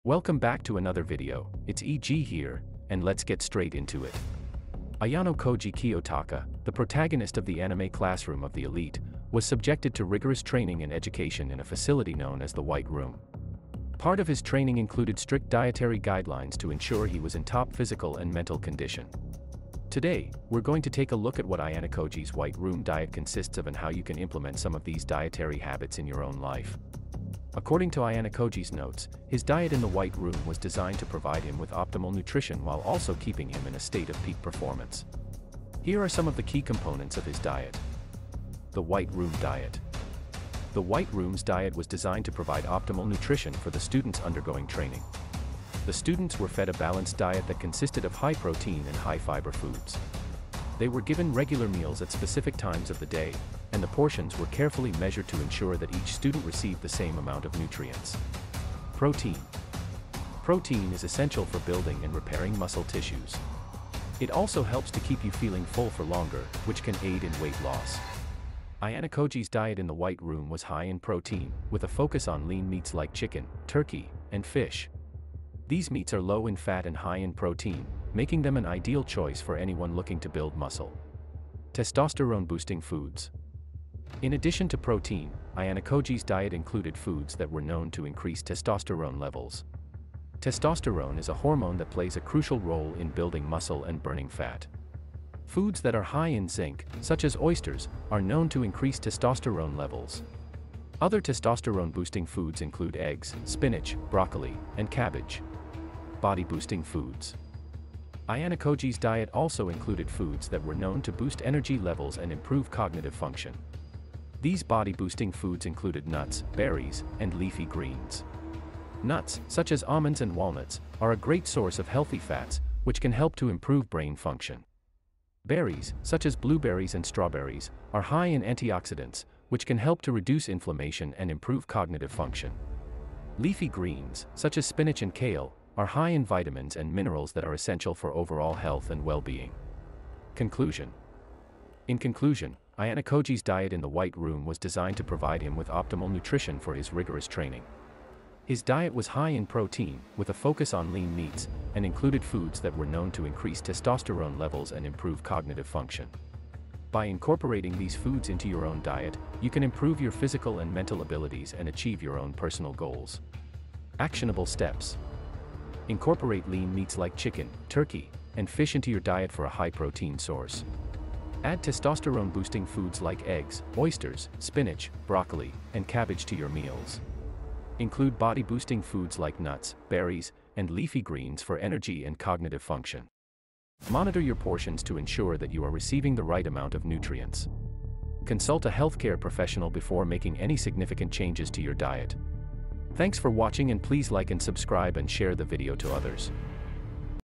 Ayanokoji's Strict White Room Diet (Vol 0)
Ayanokoji's Strict White Room Diet (Vol 0). In today's video, I break down Ayanokoji's Diet in the white room and how he gained so much muscle. High protein food, High fiber, testosterone boosting food, etc.

Chapters:

0:00 - Intro
0:15 - The Real Stuff
3:15 - Ending

How to Be Like Ayanokoji | How to Think Like Ayanokoji | How to Act Like Ayanokoji | 48 Laws of Power Ayanokoji